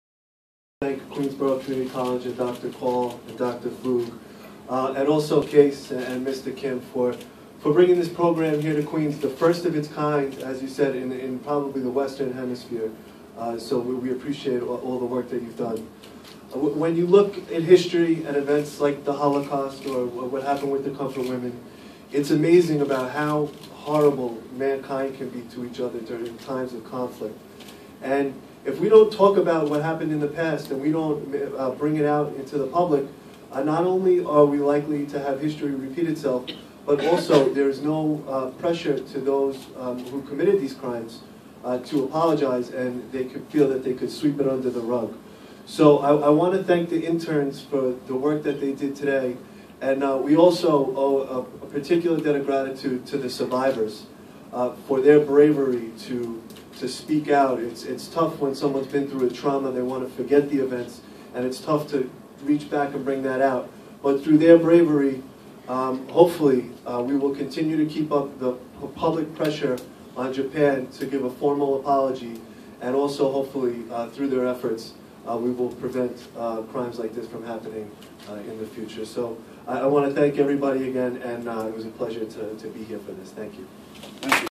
NYS Assemblyman Ed Braunstein Speaks at the Kupferberg Holocaust Center
On January 24th, 2013, the Kupferberg Holocaust Center at Queensborough Community College and the Korean American Civic Empowerment (KACE) held the Asian History Internship Presentation, showing the progress of 9 interns who interviewed 'comfort women' survivors and forced laborers under the Japanese rule.